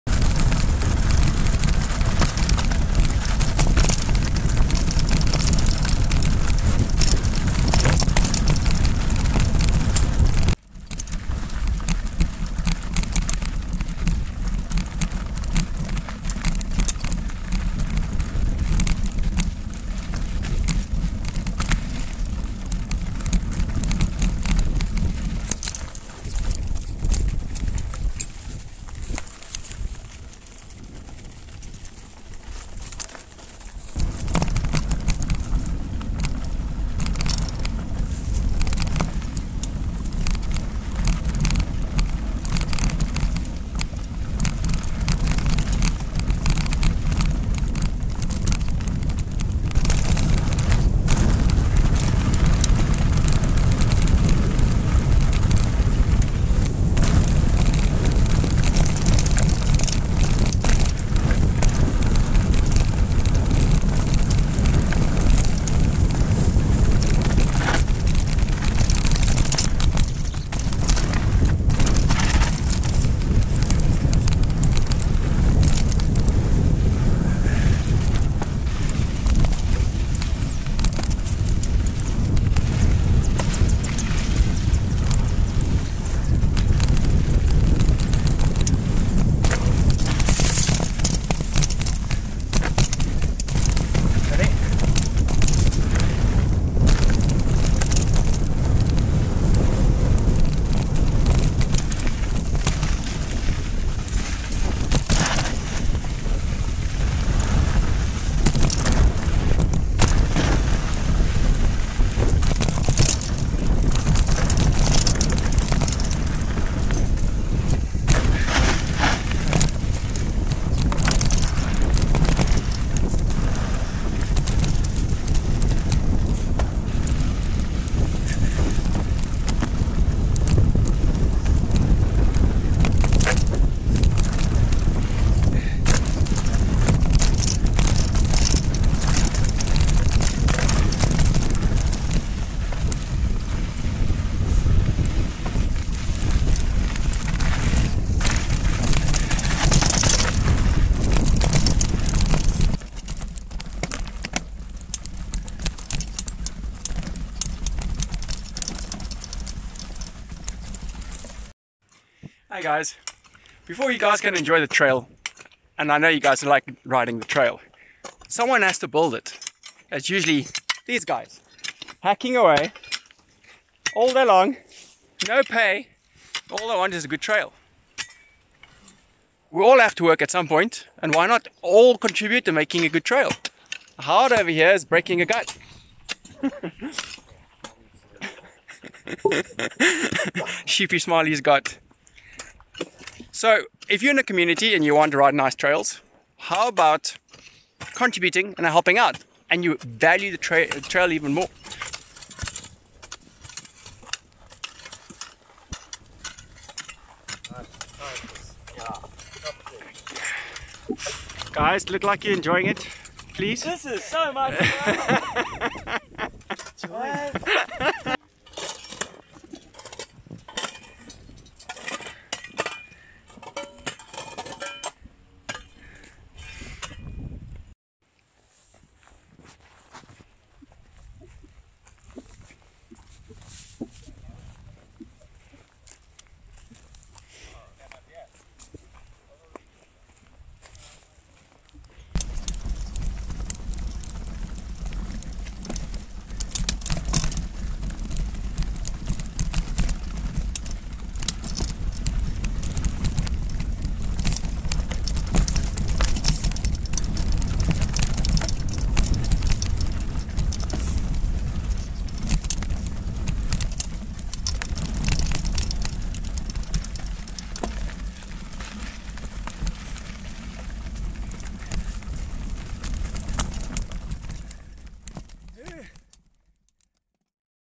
From old to new Roots
Riding the old trails of Tokai and finish off on the new rebuild Roots Trail.